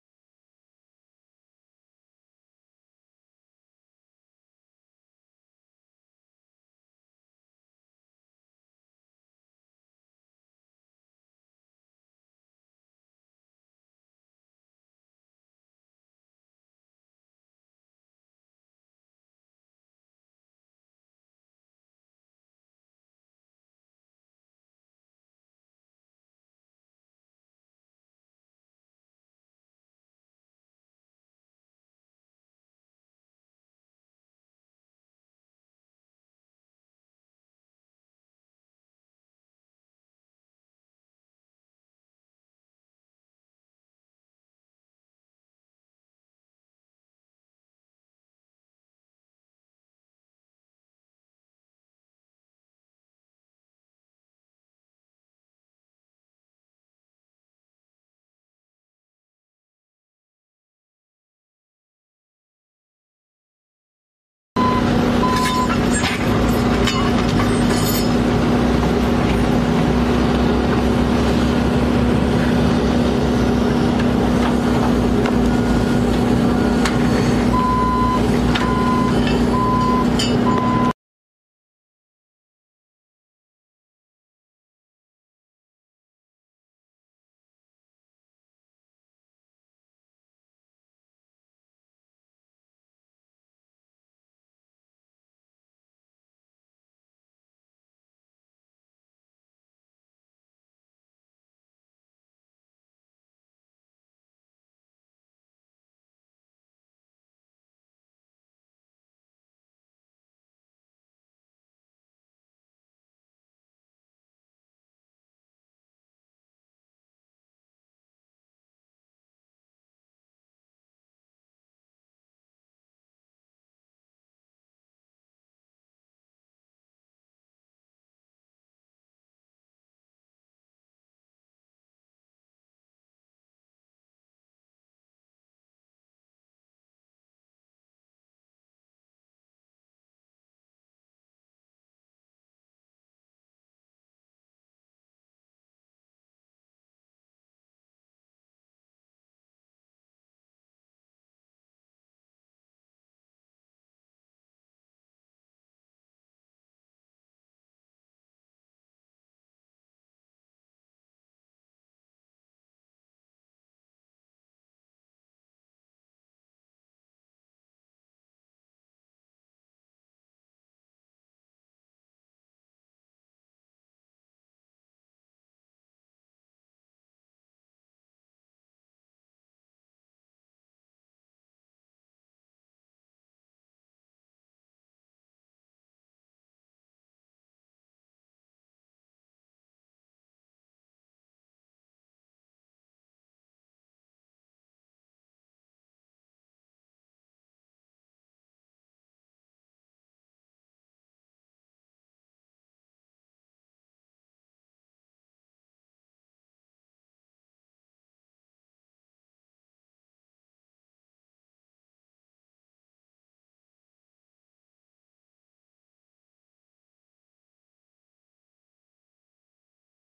U.S. Customs and Border Protection installs El Paso border wall
CBP video of border wall construction. Note: This portion of border wall was approved before President Trump's illegal and unconstitutional attempt to build his own border wall.